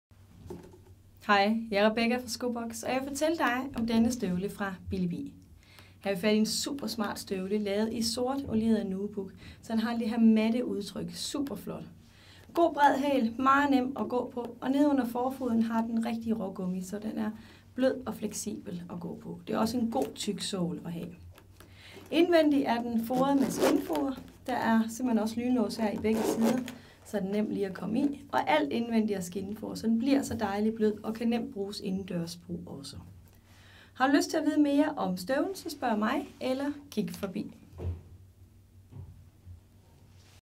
Billi Bi - Oilieret skind Støvle, Sort Nubuck støvle
Super smart og rå støvle fra Billi Bi i sort oilieret skind. Støvlen har en god længde og er skåret flot i toppen. Der er lynlås på begge sider af støvlen, så den er nem at komme i. Der er skindfoer i... læs mere på